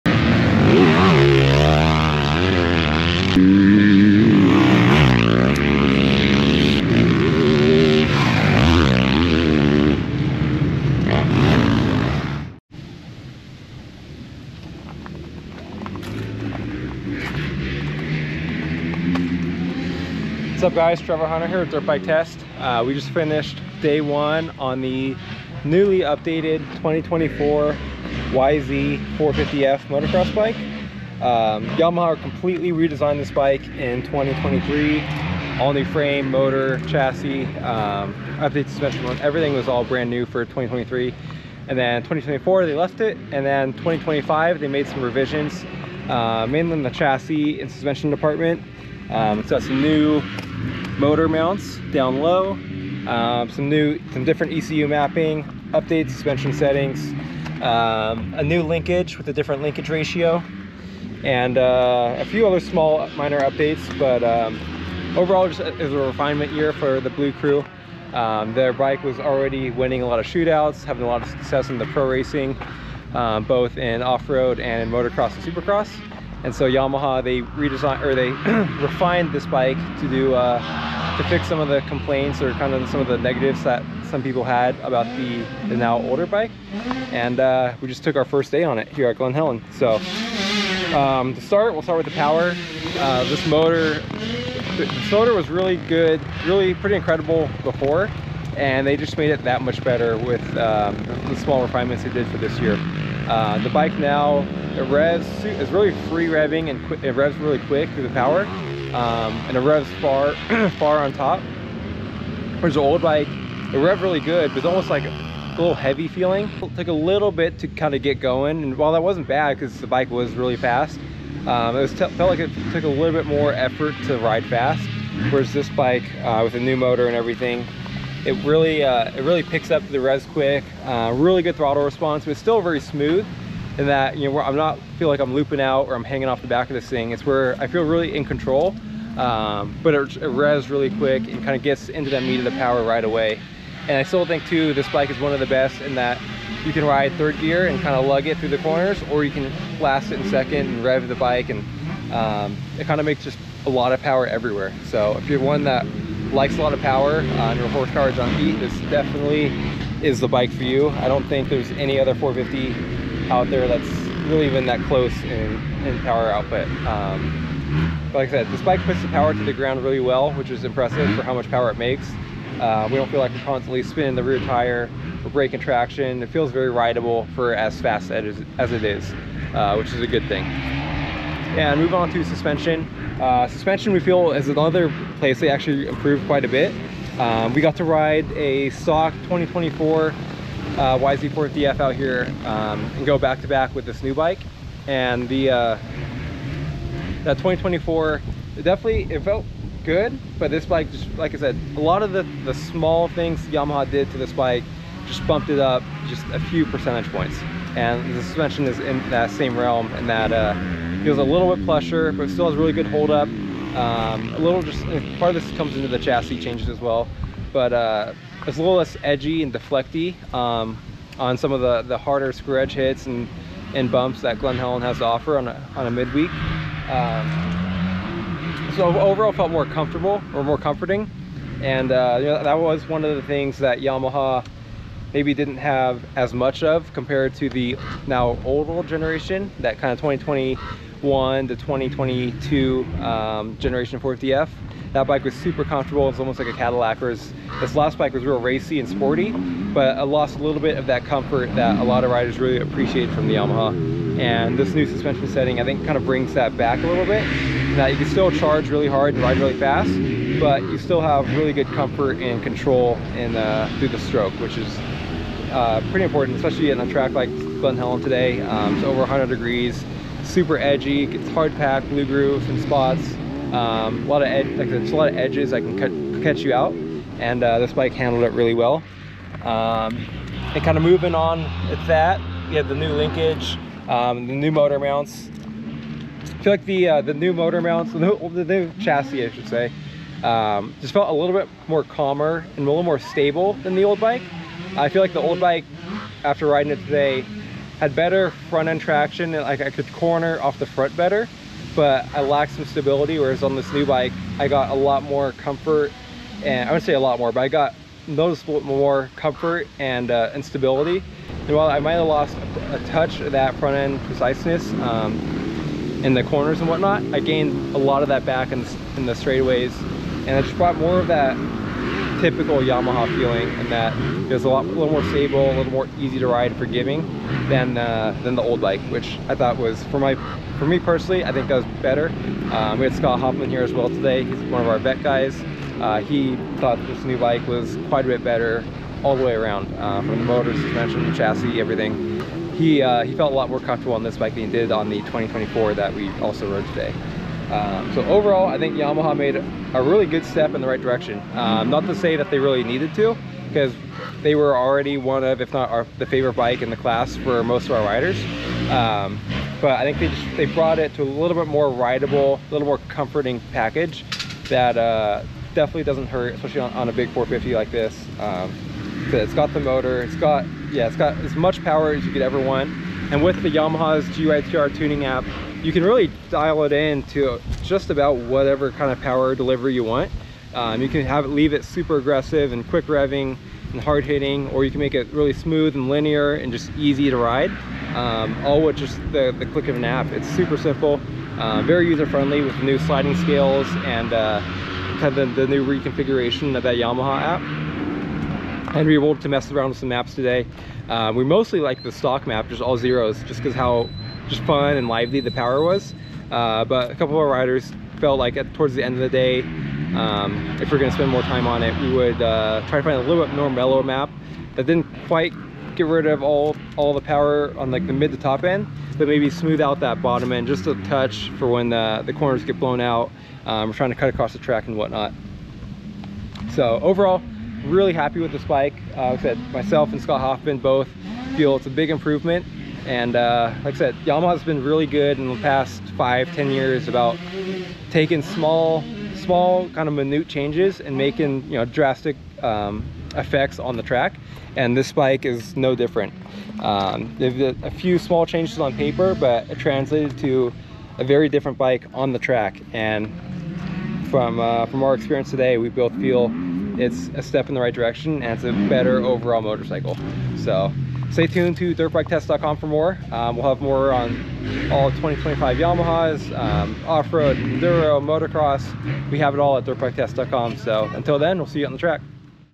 2025 Yamaha YZ450F First Impression | Dirt Bike Test
@TrevorHunter224 breaks down day one on the 2025 @Yamaha Motor USA YZ450F after a full day of testing at Glen Helen Raceway.